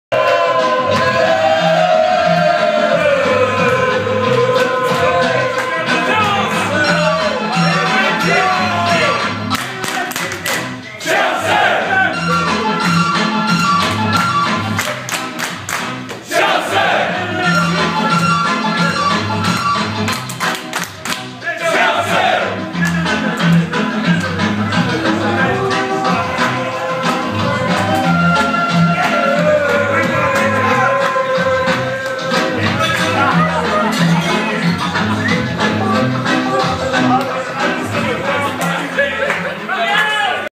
Chelsea FA Cup Celebration Mad Dog SF
Celebrating triumph of Chelsea FC in FA Cup final vs Liverpool at Mad Dog in the Fog Pub in San Francisco with a little music